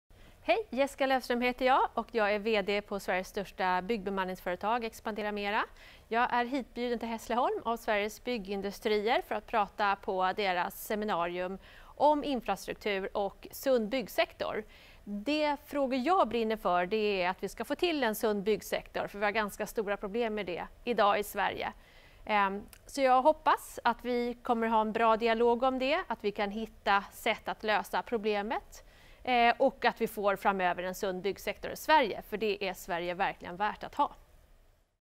Intervju Jessica Löfström, VD ExpanderaMera
Intervju med Jessica Löfström under Sveriges Byggindustriers infratoppmöte i Hässleholm 12 januari 2018.